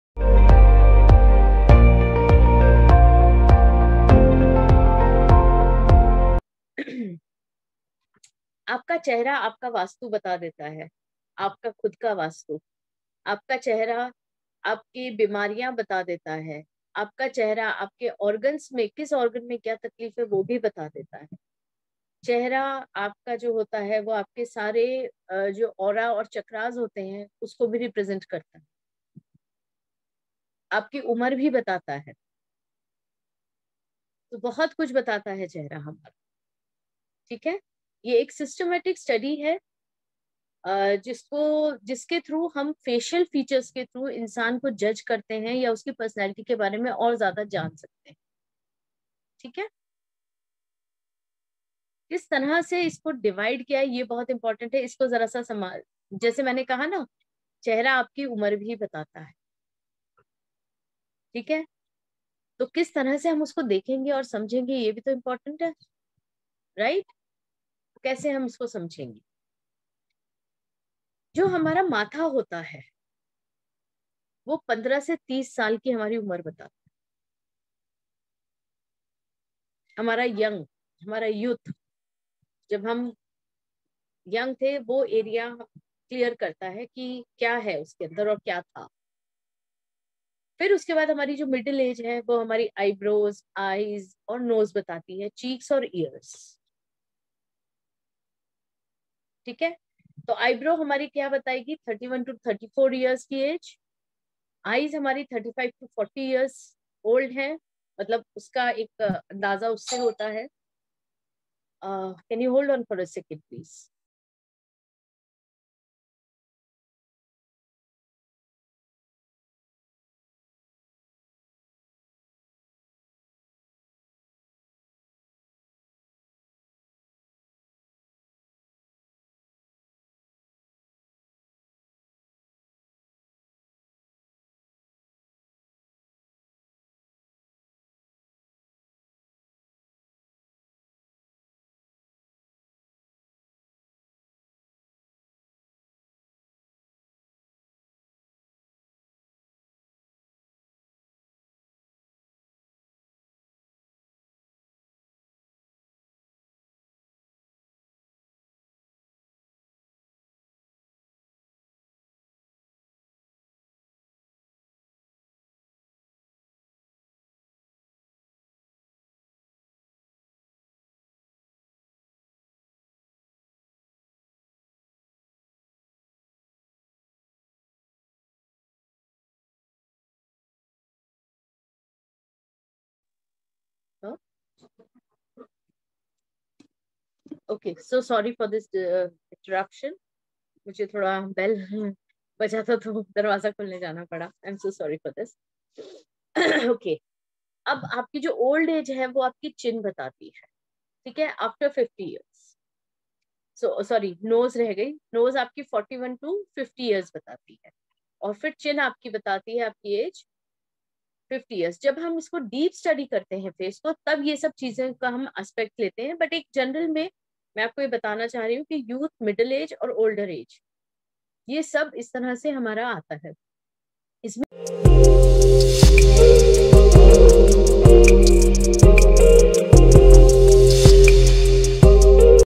Learn about Face Reading
Topics of Face Reading Course.
👉Reading the face:
* Eyes
* Eyebrows
* Nose
* Lips
* Ears
 👉 Understanding different emotions...
*  Happiness
* Sadness
* Surprise
*  Fear
* Disgust
* Anger
👉Evaluation of situations
* Read, catch n understand  Micro expressions
👉 Pair expression with body language.